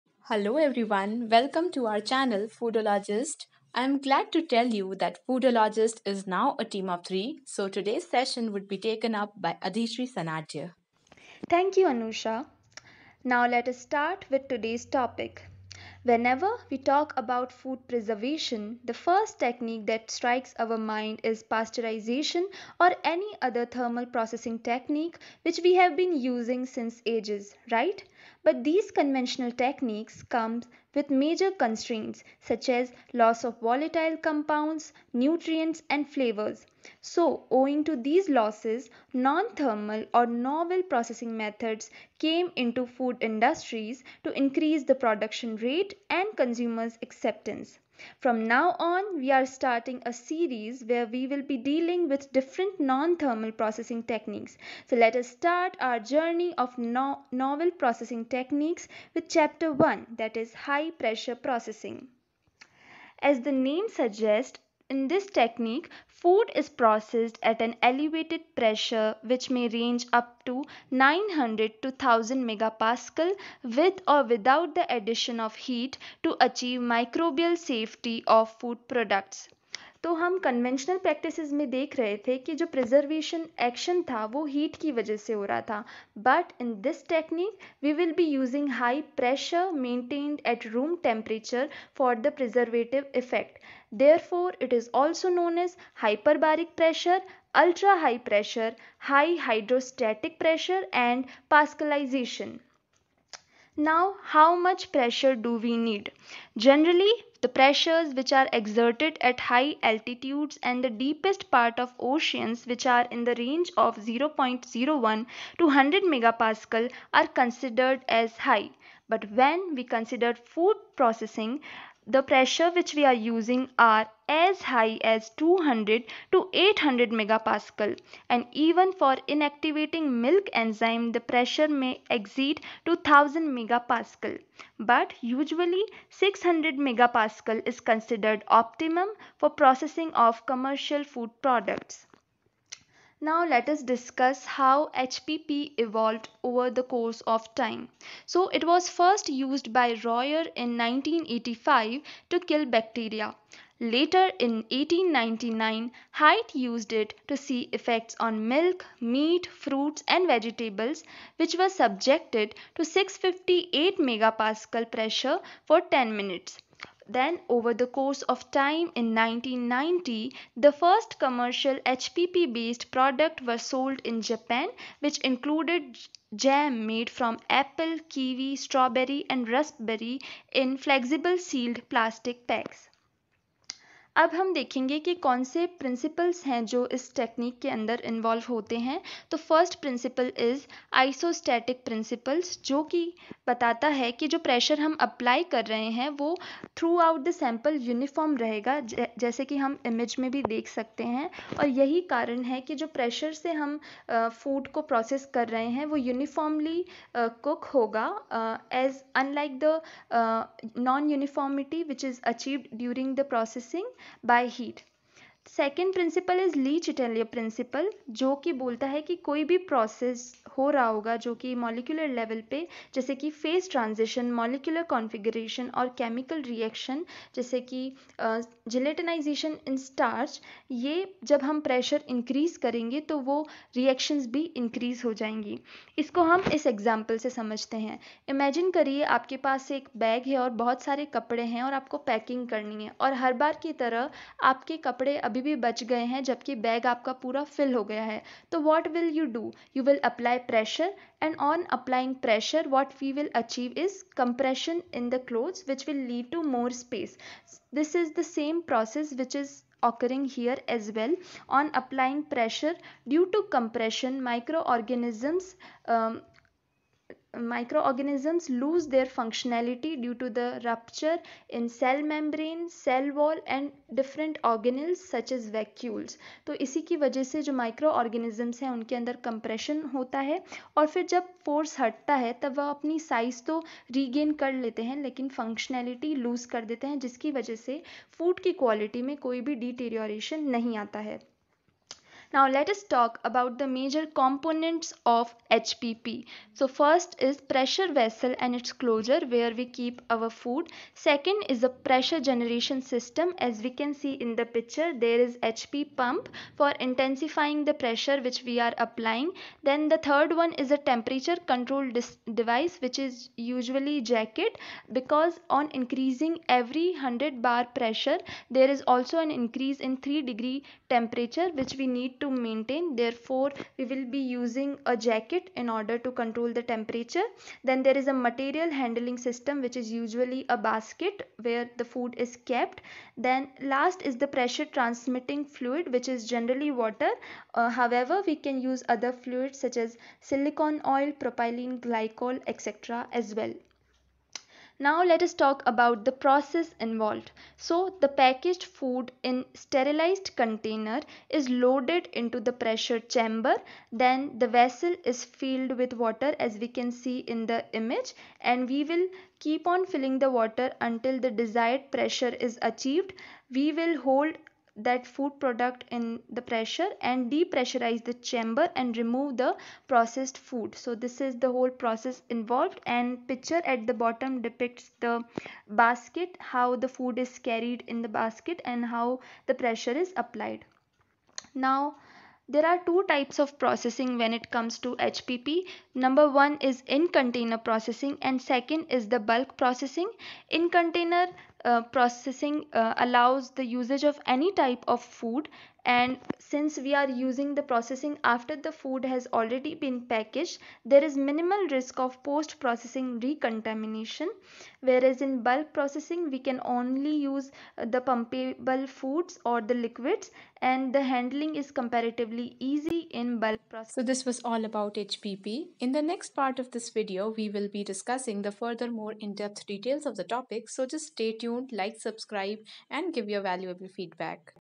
High Pressure Processing l part 1 by Adhishree Sanadhya novel non thermal
High-pressure processing (HPP) is a non-thermal method of preservation and sterilization of food products, in which a product is subjected to very high pressure, leading to the inactivation of certain microorganisms and enzymes in the food. It is a novel alternative to the thermal processing in the food industry and allows food to be pasteurized at or near room temperature. High pressure (HP) treated foods have most of the “fresh like” attributes as it enables microorganisms to be destroyed with little or negative effects on flavour components, colours, vitamins, and other nutrients, hence, does not hamper the nutritional and organoleptic properties of the product, thus offering considerable advantages as compared to thermal processing.
Also known as cold pasteurization technique by which products, already sealed in their final package, are introduced into a vessel and subjected to a high level of isostatic pressure usually transmitted by water.